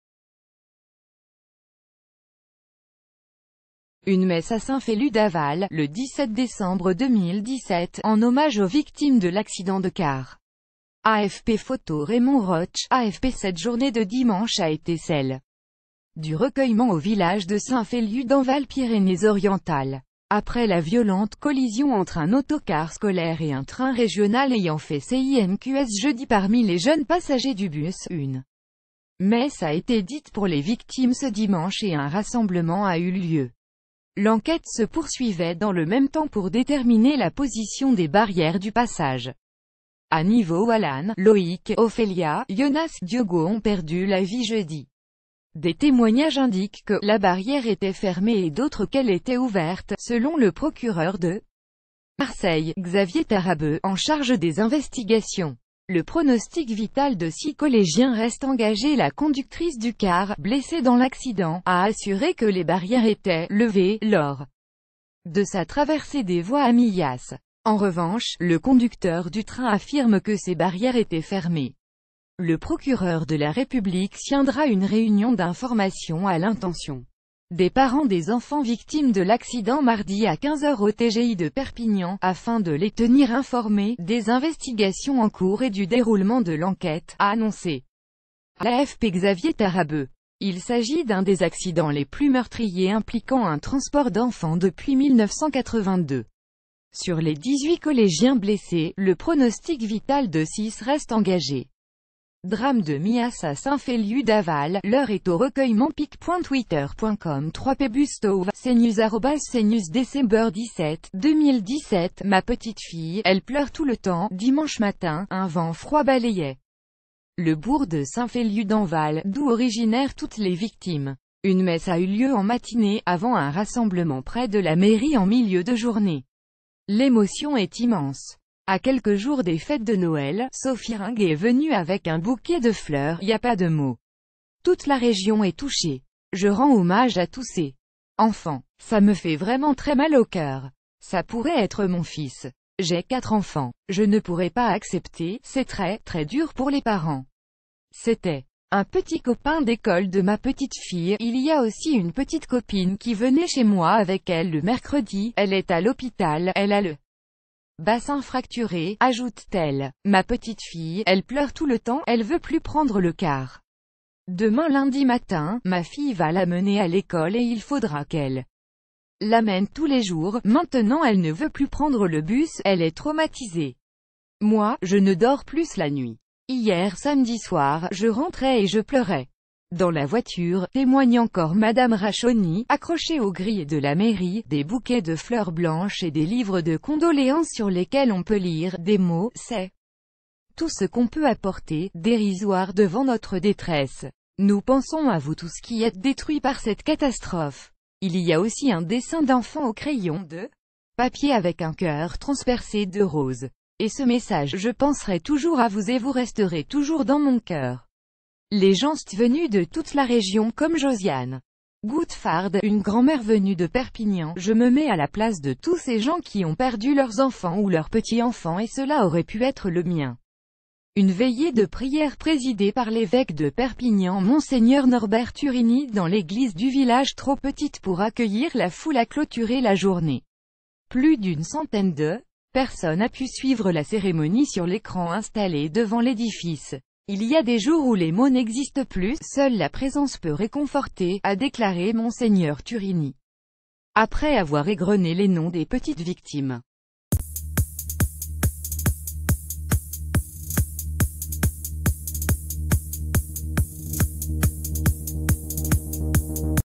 Accident de millas: le village de saint-féliu-d'aval se recueille dans la douleur
Une messe à SaintFeliu dAvall, le 17 décembre 2017, en hommage aux victimes de laccident de car. AFP PHOTO RAYMOND ROIG — AFP Cette journée de dimanche a été celle du recueillement au village de Saint...